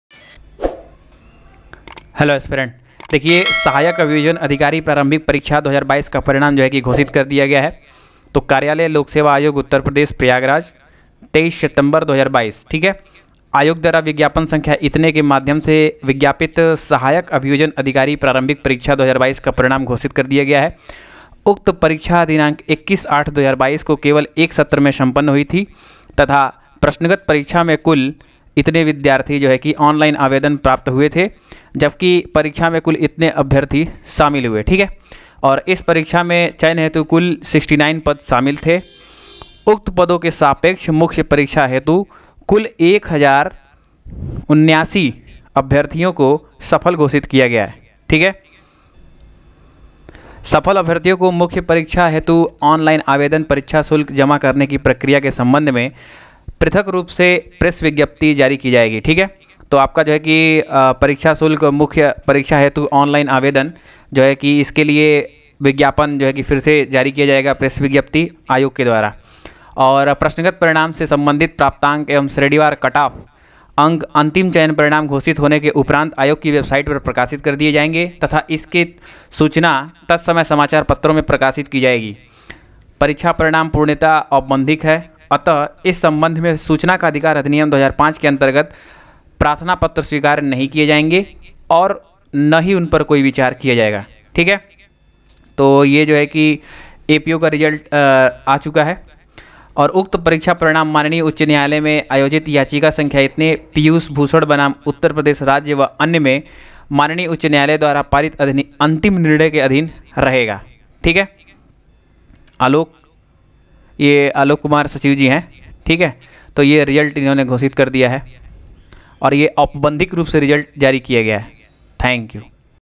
UPPSC:सहायक अभियोजन अधिकारी !uppsc
👉NOTE:
1- The Result of the Female candidates who are from out of U.P. shall be
subject to final order and judgment of Hon’ble High Court in Special Appeal

👉UPPSC सहायक अभियोजन अधिकारी (APO) प्री रिजल्ट जारी 💐
मेन्स के लिए कुल - 1079 अभ्यर्थी पास